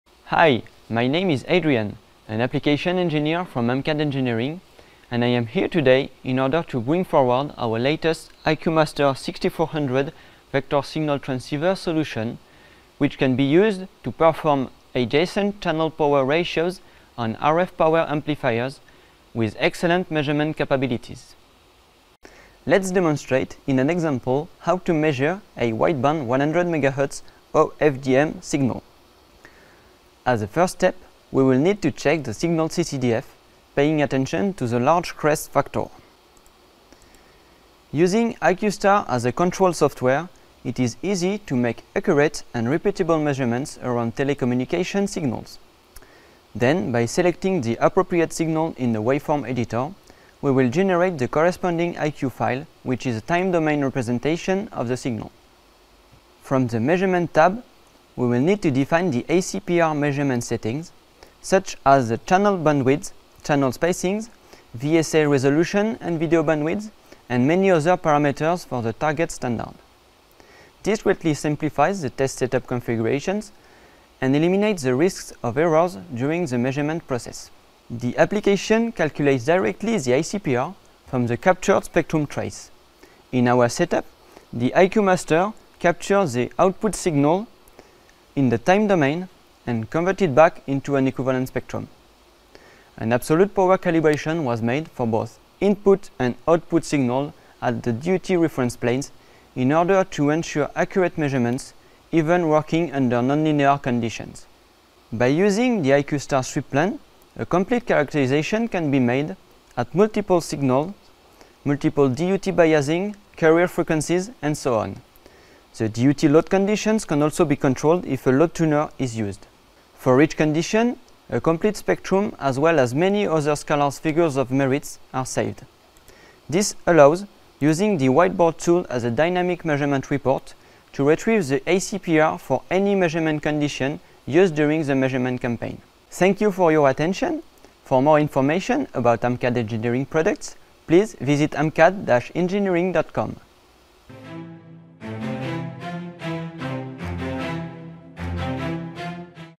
Adjacent Channel Leakage Ratio (ACLR) Measurements.
Our IQMASTER 6400 Vector Signal Transceiver solution, can be used to measure Adjacent Channel Power Ratios on RF Power Amplifiers with excellent measurement capabilities.
Let’s demonstrate in an example how to measure a wideband 100 MHz OFDM signal.